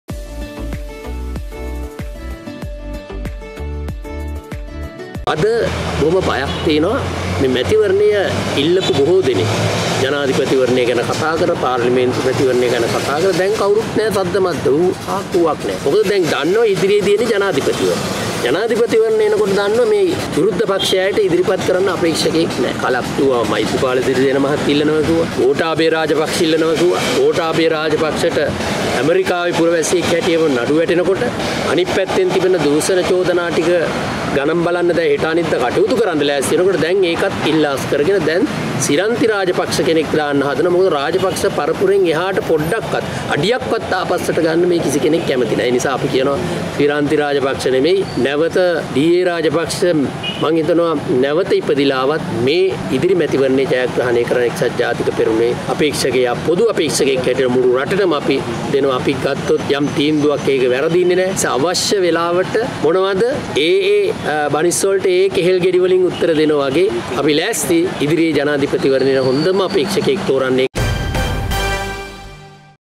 ඩී.ඒ.රාජපක්ෂ මහතා නැවත ඉපදුනත් ඉදිරි මැතිවරණ ජයග්‍රහණය එජාප සතුයි - විජේපාල හෙට්ටිආරච්චි
UNP will win even if D.A. Rajapaksa is reborn – Wijepala Hettiarachchi